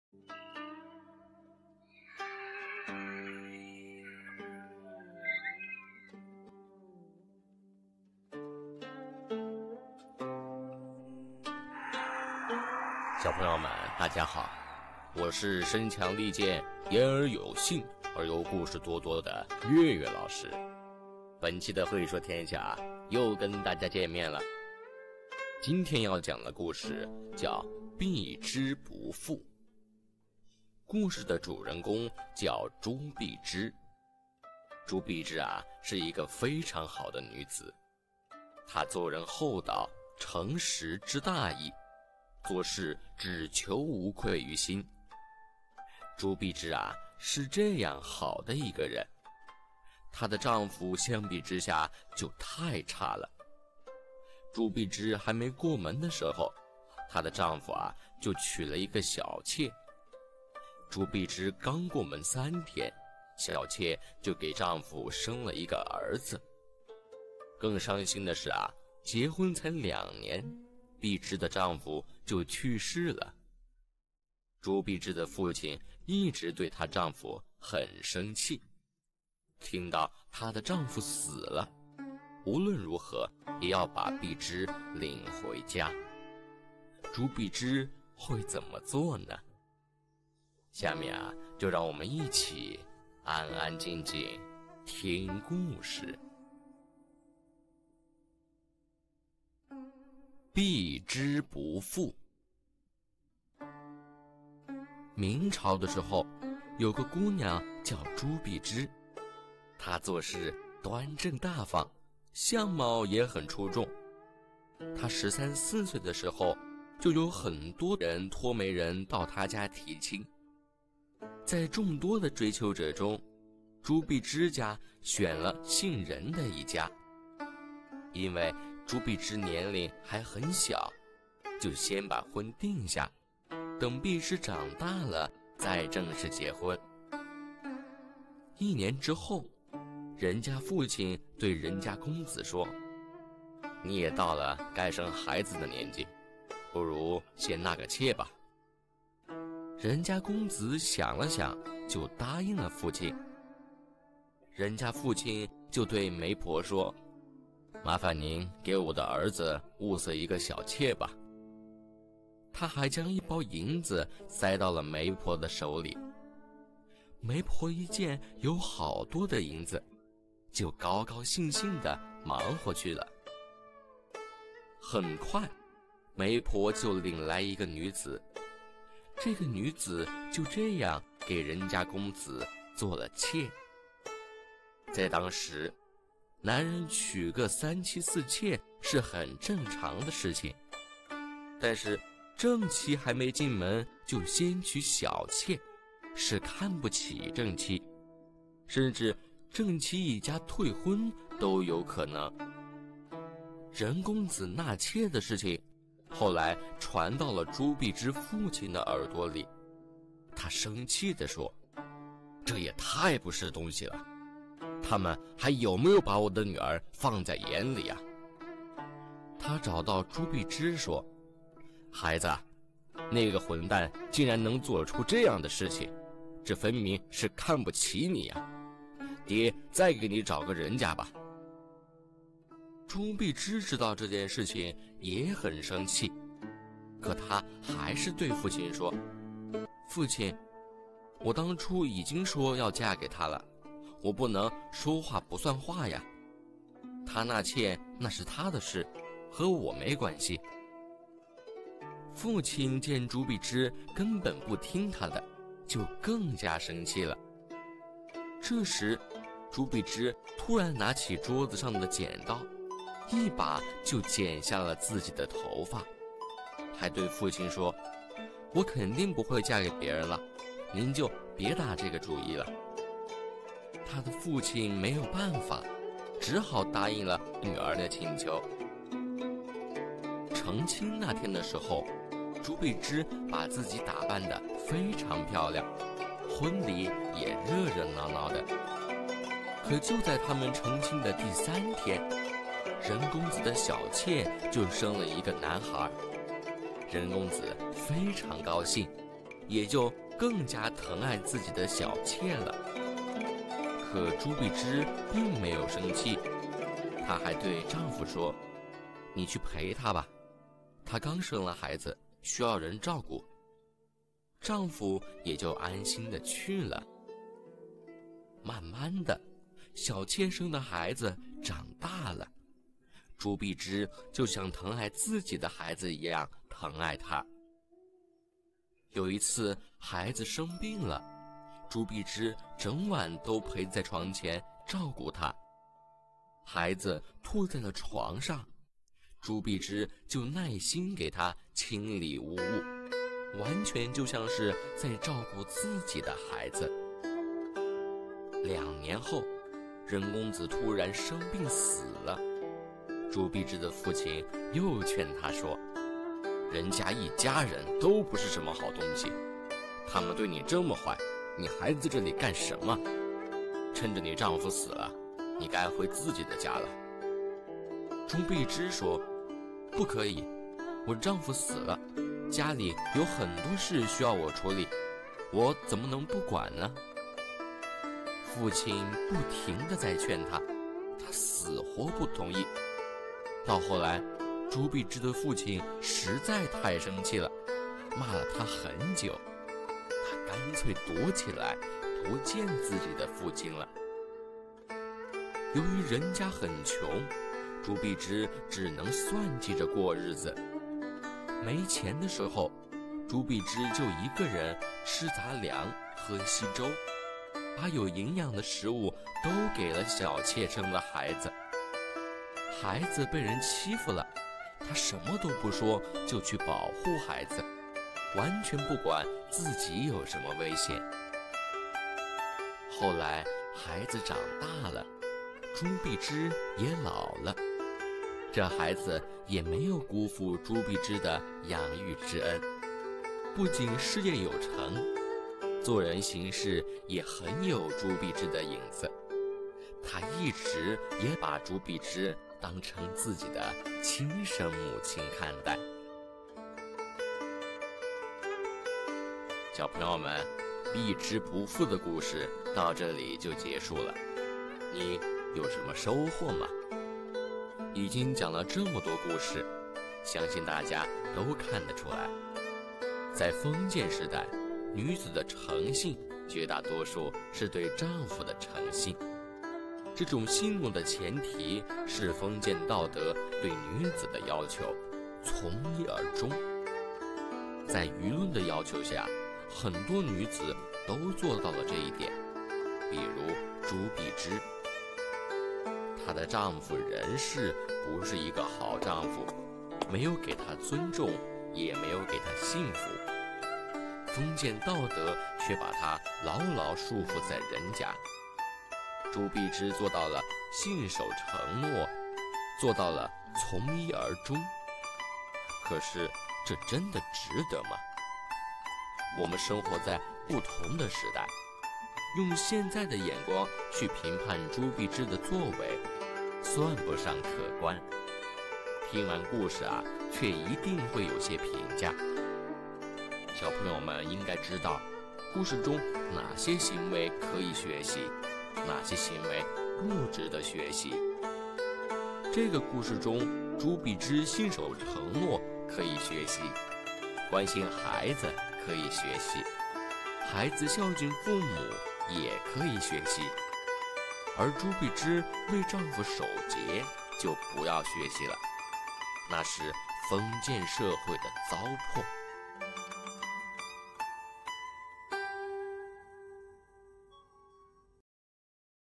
167璧枝不负|国学|中华民族传统美德故事277集|诚信为本篇72
明朱璧枝。未婚誓守。既嫁夫亡。终身不负。

   【原文】

   明任某、聘朱璧枝。未婚先纳妾。朱父怒。欲渝盟。璧枝翦发自誓。父不得已。仍归任。婚三日。妾生子。璧枝爱如己出。越二年。任亡。父唁之曰。任子负尔甚矣。尔何处不可容身。璧枝曰。宁彼负儿。儿不可负彼。父怒骂之。欲携归。璧枝逃匿不出。食贫力作。抚妾子终其身。

   任某未婚先纳妾。朱父怒欲渝盟。人之常情。婚三日。妾生子。居二载。夫又亡。朱父唁之。璧枝以不可负对。父怒而詈之。欲携以归。乃竟逃匿不出。食贫力作。抚妾子以终身。其信德尚矣。

   【白话解释】

   明朝有个姓任的人，同朱璧枝订了婚，没有娶过门来，就先办了一个小老婆。朱璧枝的父亲得知了这件事，很生了气，欲把朱璧枝另外再许人家。朱璧枝就翦下头发发了誓，一定不可。他的父亲没有办法，仍旧同任家结婚。结婚以后，过了三天，任家的小老婆，就生了一个儿子。朱璧枝就当着自己生的一样爱惜，过了两年，姓任的死了。朱璧枝的父亲就劝他说：“任家这个人对你不住，达于极点了。你那里地方不可以安身，何必这样好心对他呢？”朱璧枝说：“宁可他来对不住我，我却不可以对不住他。”朱璧枝的父亲就发了怒骂他，一定要同他回去，朱璧枝就逃开躲起了不走出来。后来因为任家里很穷，尽力操作过着日子，把小老婆生的儿子抚养到长大才了。